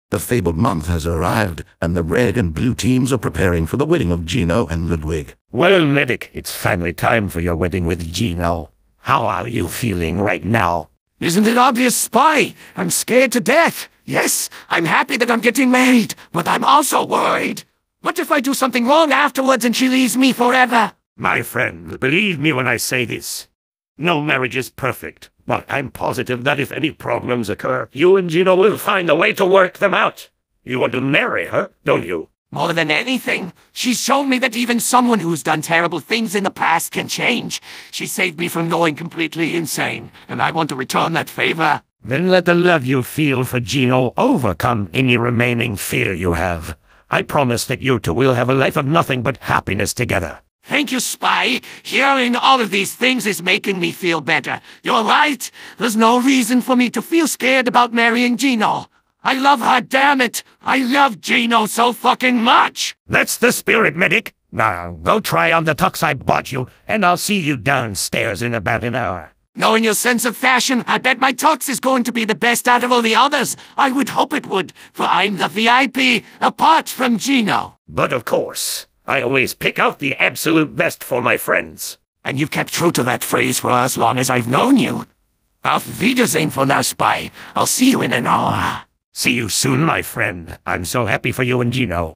[TF2 15.ai AU] Origins: How Medic Und Geno Met (Part 6) {Zhe Vedding} Teaser
I'm so sorry it took two months before I came back to zhis series. I had ozher ideas planned und also vanted to vait until I got 900 subscribers to do zhe series finale. Untill zhe full video comes out, enjoy zhis short preview!

I have no ownership over the pictures featured in this video. I found them off of Discord and Google. They belong to their respective artists.

Team Fortress 2 belongs to Valve.